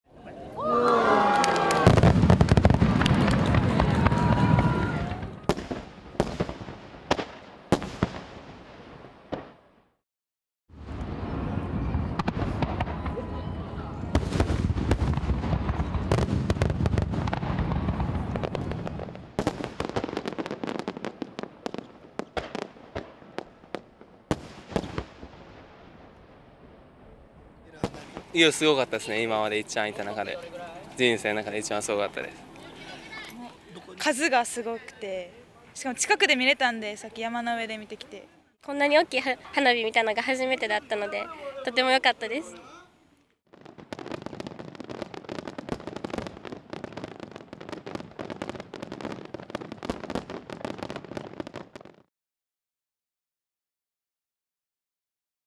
１万発が夏の夜空彩る 大阪でＰＬ花火
大阪の真夏の風物詩「ＰＬ花火芸術」が１日夜、大阪府富田林市のＰＬ教団大本庁で開かれ、約１万発の花火が夜空を彩った。今年は２０カ所から幅約２００メートルにわたって色とりどりの花火を打ち上げた。